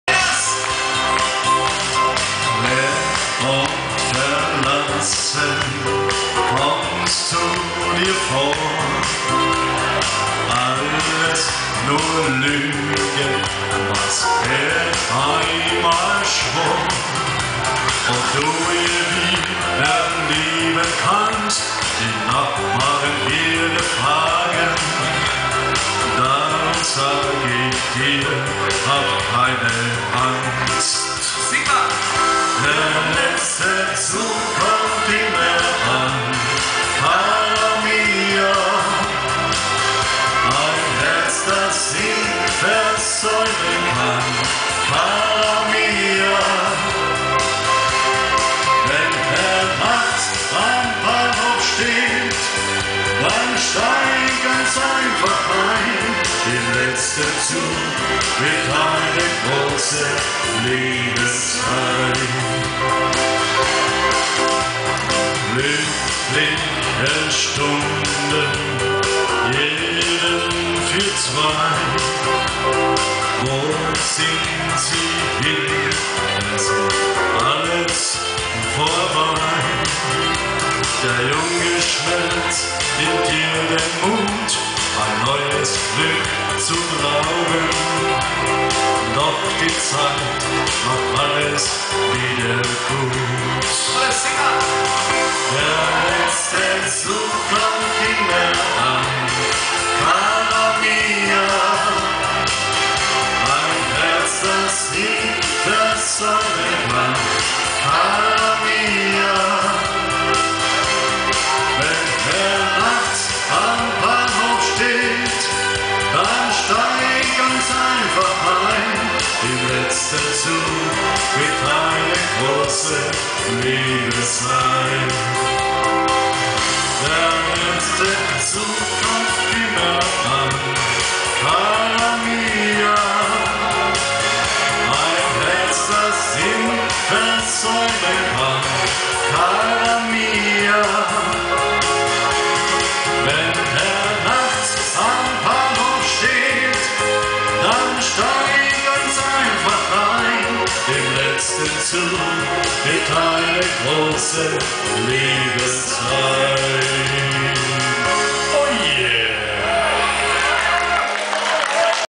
der letzte Zug.MPG
die 3 Wendlbuam am St. Veiter Wiesenmarkt, Lobnig Walter Solist und Kapellmeister der Trachtenkapelle Griffen.